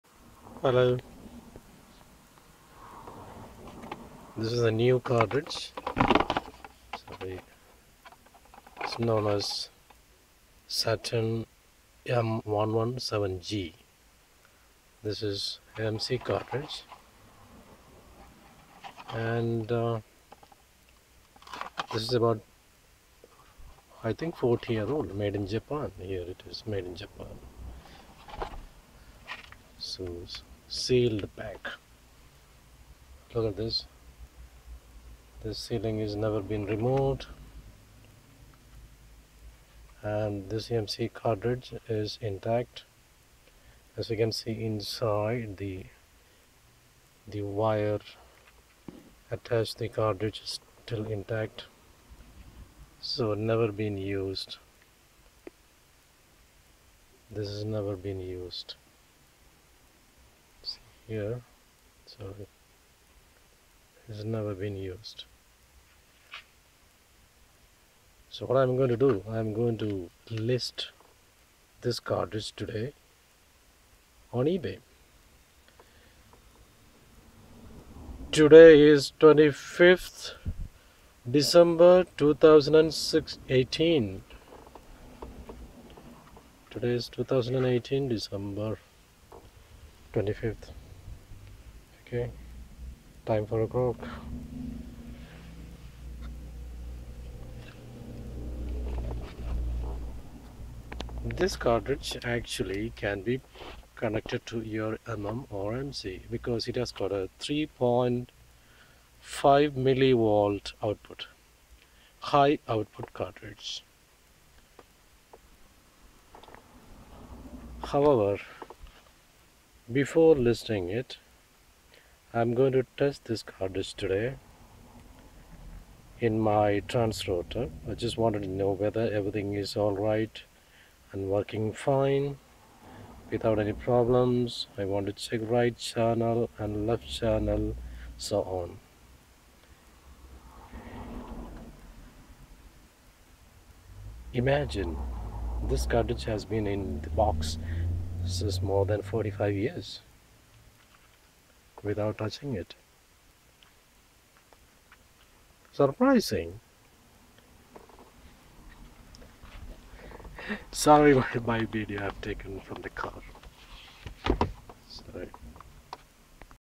new Satin cartridge Moving Coil MC model M-117G
Description for sale on eBay .Mc cartridge.M-117G.tested sounds brilliant.date of purchase 24/12./2018.